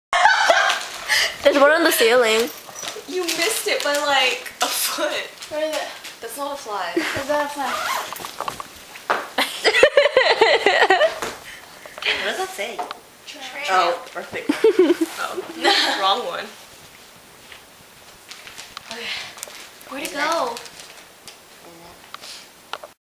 Killing A Fly Part 2
September 14, 2008. During Thy's 18th birthday party sleepover. there were like 10 flies in the house because the backyard door opened and closed frequently during the party.

There's the word "tramp" on thy's lower back because it represents a "tramp stamp" lol. courtesy of tess and tipp